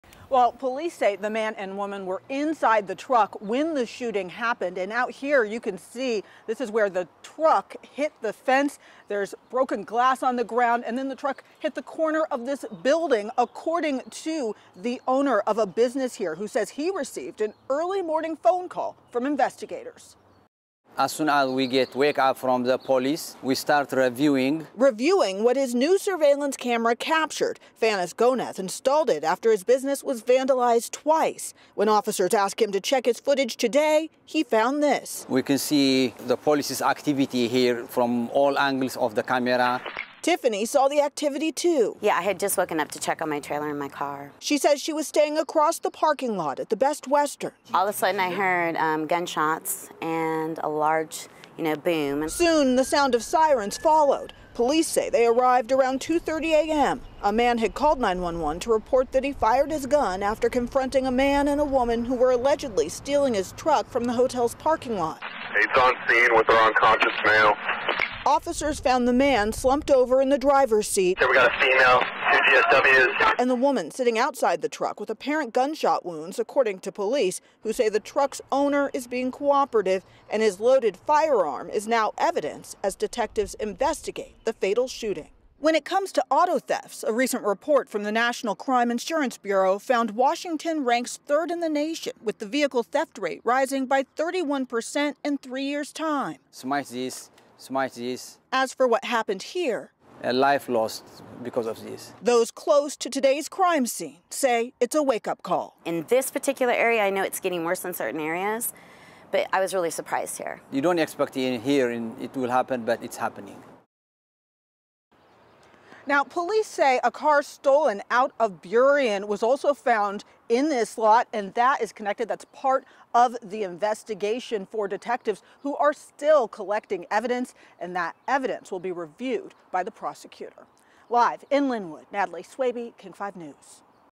Man shoots 2 people allegedly trying to steal truck in Lynnwood; 1 dead
One man died and a woman was injured after they were shot by a man who claims the two were trying to steal his truck in Lynnwood early Tuesday morning.

Just after 2:30 a.m.,  a caller to 911 reported he had fired his gun after confronting a male and female in the act of stealing his truck from the parking lot of the Best Western Alderwood. Lynnwood police officers arrived and found a truck resting against the corner of a building and a man slumped over in the driver's seat. A woman was sitting outside the truck with gunshot wounds.